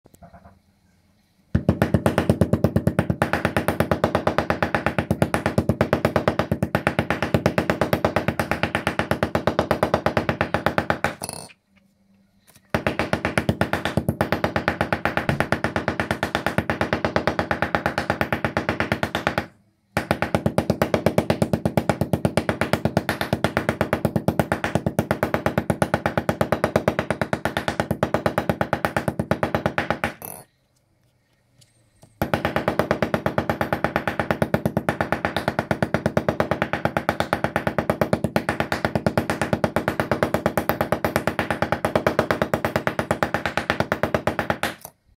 How to pound 1-minute-steak - minute steak
How to make and pound one minute steak. Easy to do. Everyone can do. Homemade food for everyone.